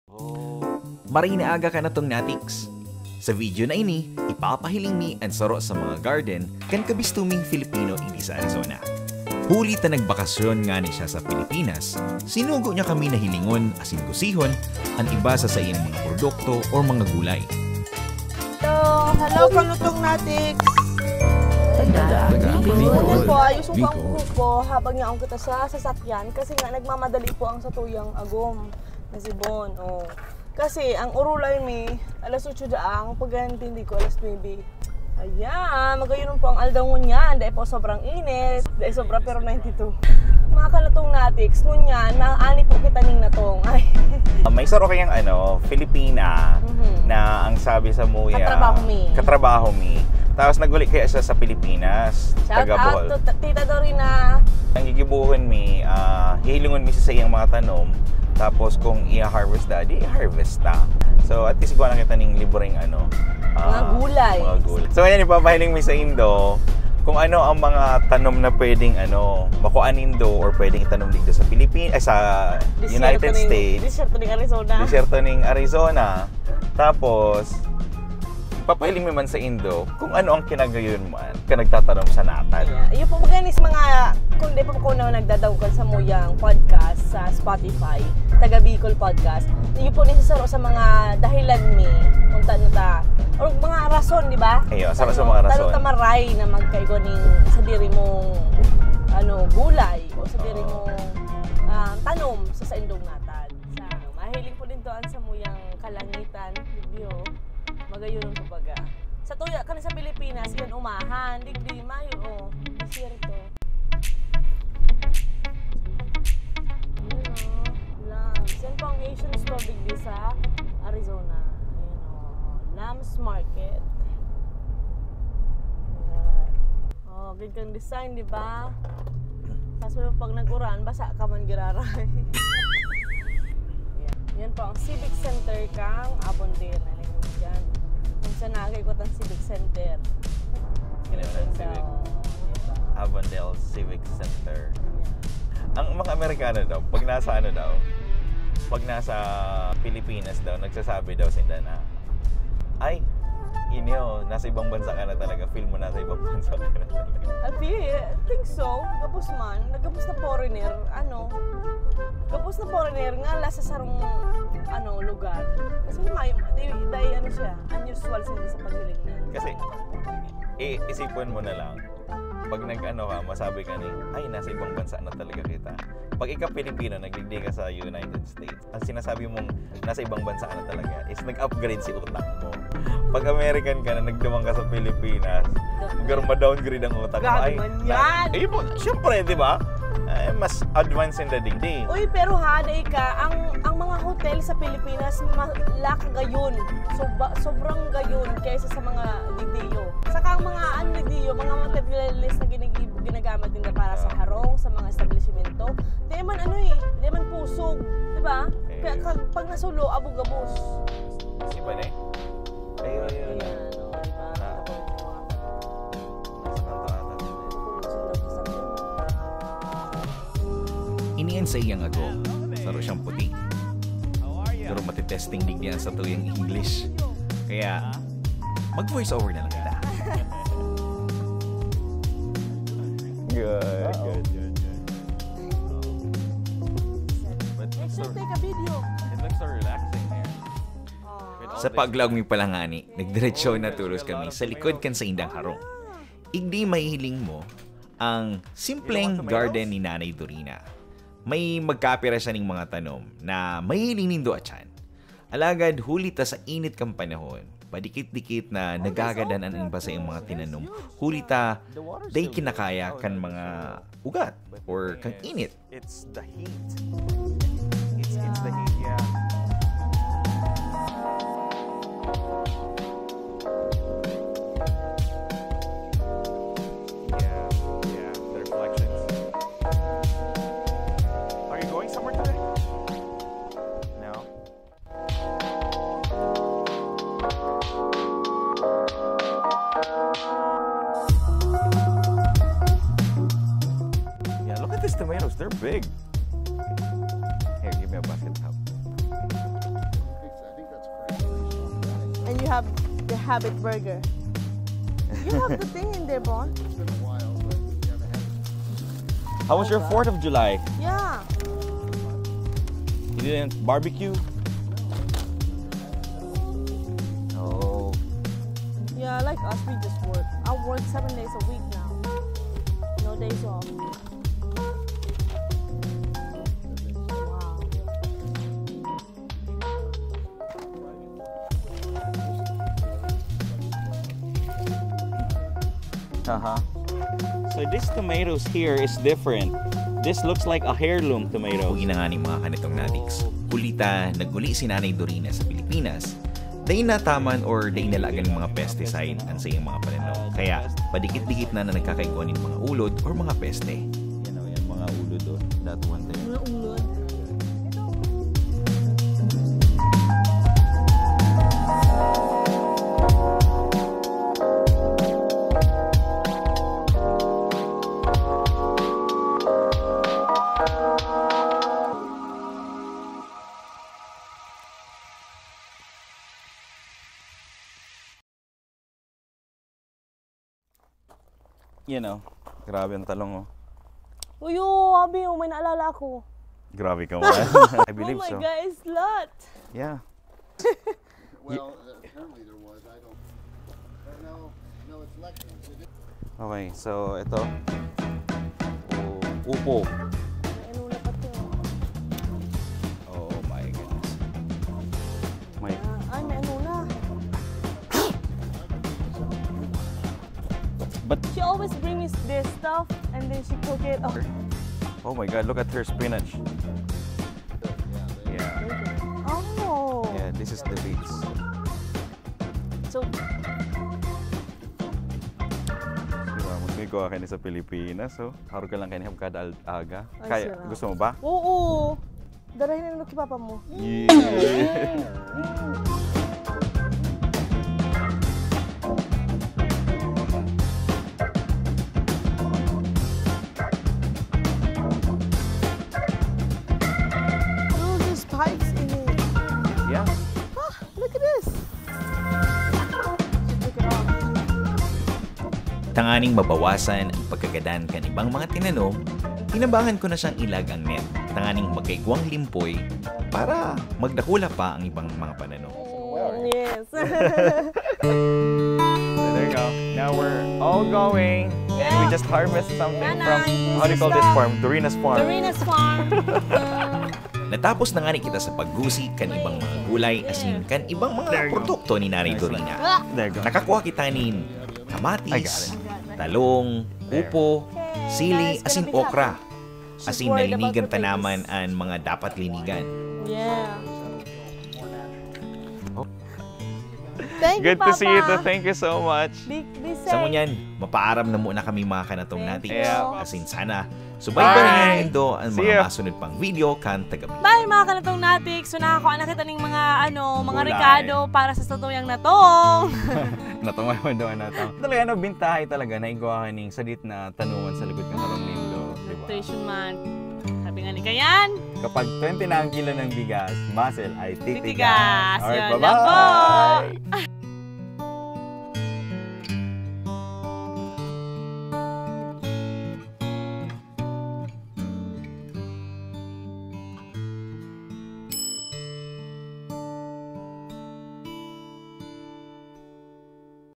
Growing a Filipino Garden in Arizona by Taga Bikol
Taga Bikol visits a Filipina house with small garden in her backyard. She grown some plants in Arizona before summer and will see how plants survive in the middle of the heat.

Follow our podcast and other content on social media.